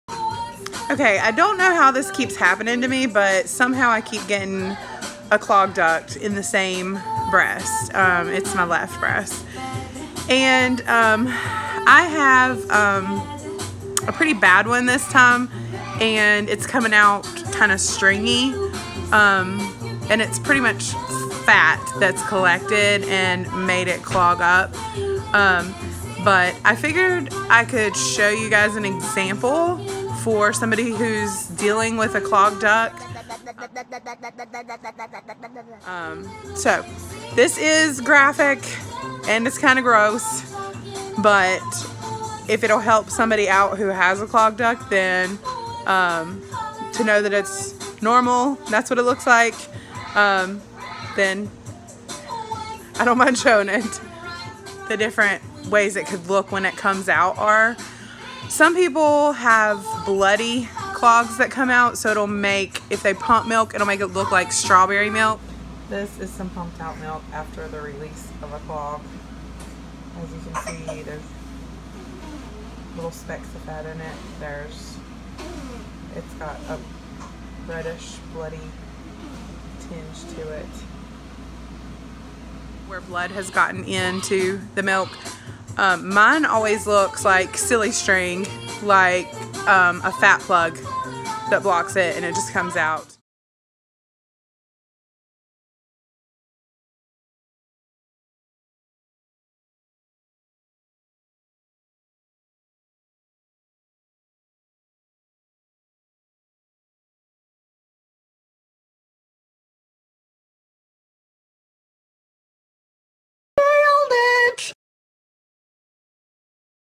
What a clogged milk duct looks like
This is images of what my clogged milk duct that I’ve been battling today looks like.